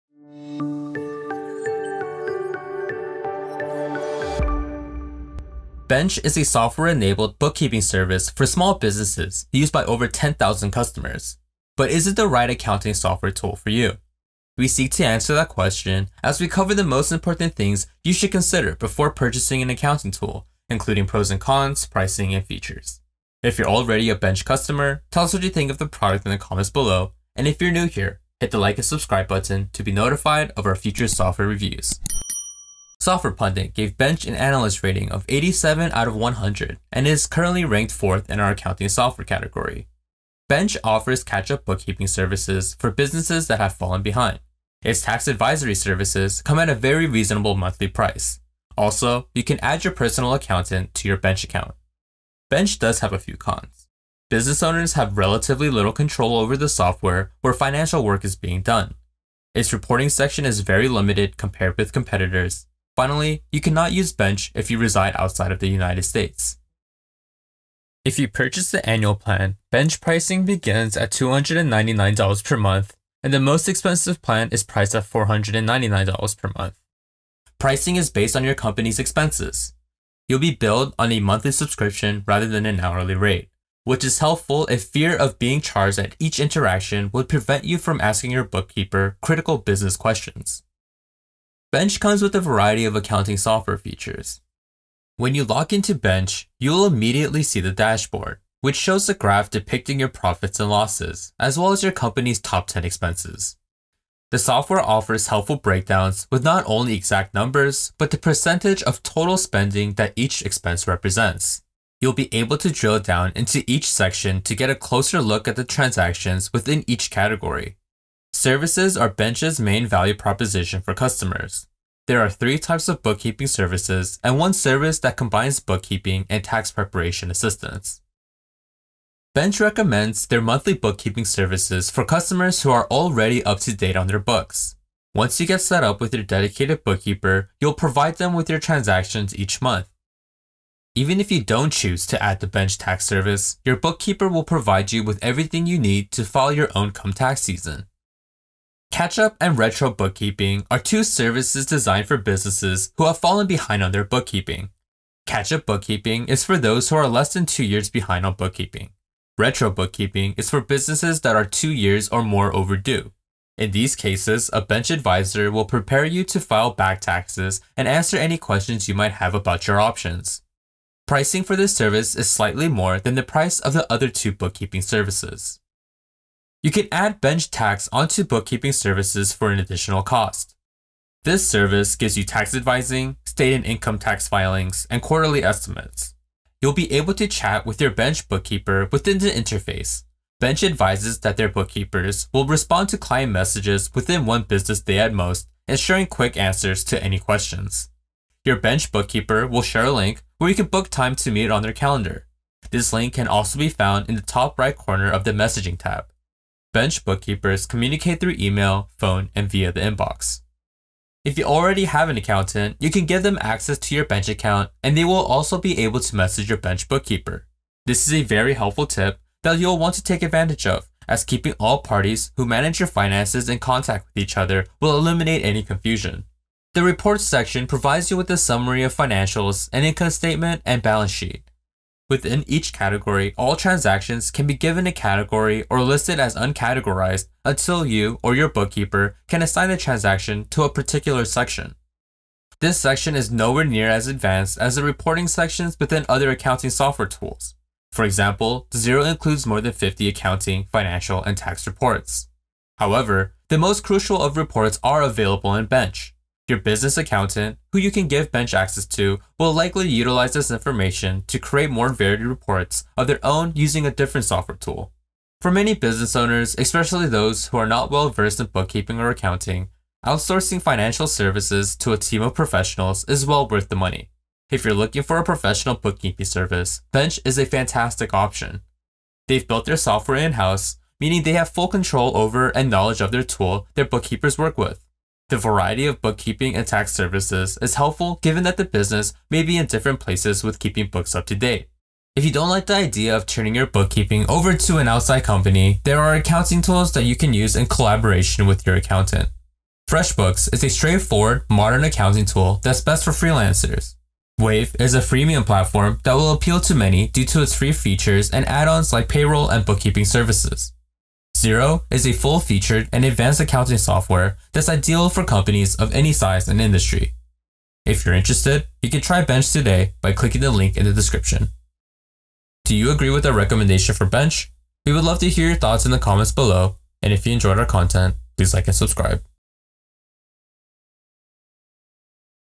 Bench Review
Bench is a software-enabled bookkeeping service for small companies. With three different bookkeeping service options, as well as a tax service, Bench is a great option for small businesses that want a hands-off approach to bookkeeping.

This step-by-step walkthrough of the product includes details about pricing, pros and cons, and an analysis of Bench’s major features.

⏰TIMESTAMPS ⏰
0:00 Introduction
0:33 Rating
0:40 Pros and Cons
1:20 Pricing
1:31 Dashboard
1:58 Services
3:06 Bookkeeper Communication
3:46 Reports
4:25 Is Bench Right For You?